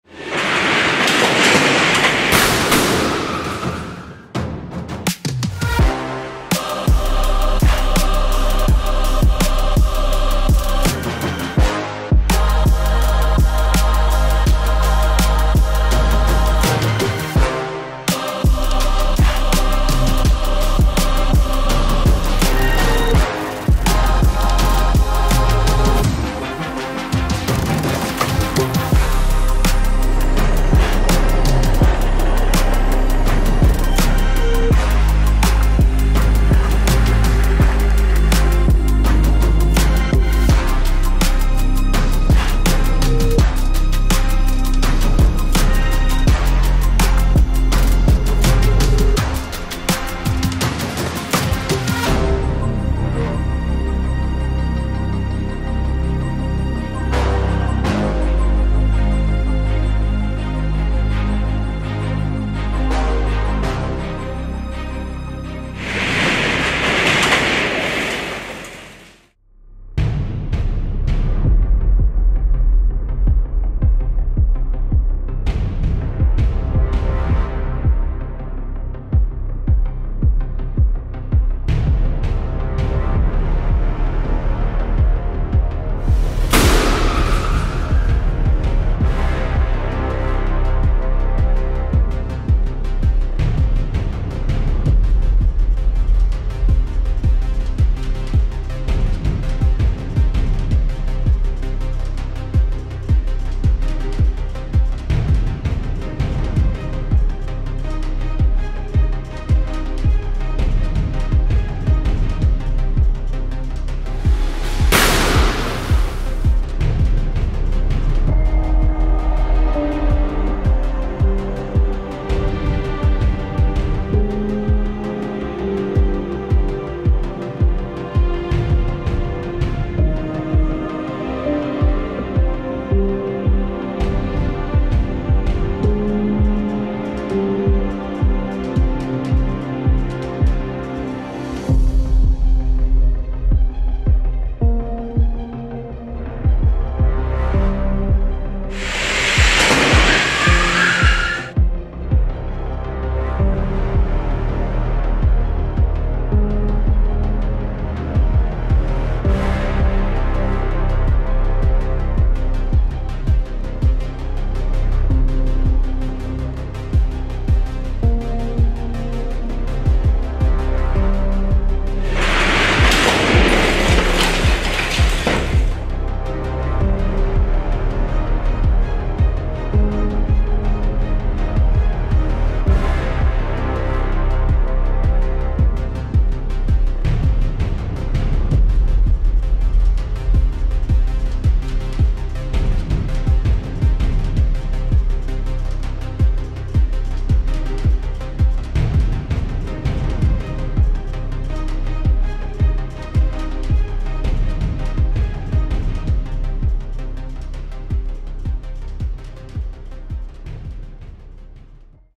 Toyota Yaris Cross - Crash and Safety Test
The Toyota Yaris Cross follows hot on the heels of the Mirai and Yaris with another 5 star rating.

The car, available at petrol and HEV petrol, was tested partly in Australia and Europe, two important markets for the Yaris Cross. The result is published in collaboration with sister organisation ANCAP.

✅ Source: Euro NCAP